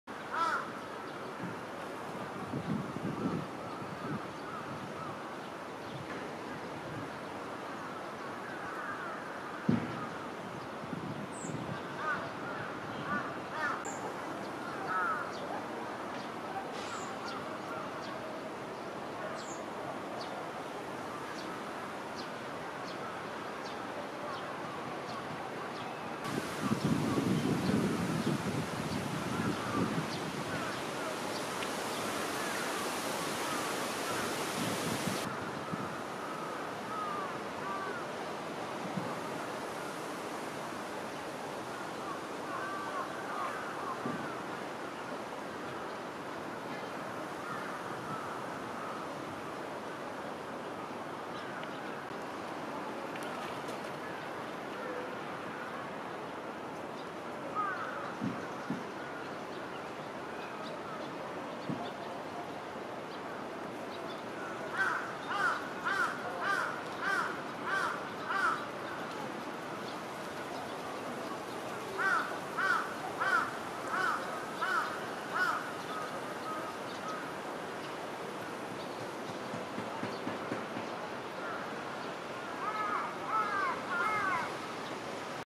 Oriental Turtle Doves
Oriental Turtle Doves  -  Streptopelia orientalis of the nominate race S. o. orientalis.

Central Tokyo , Japan, April 2015,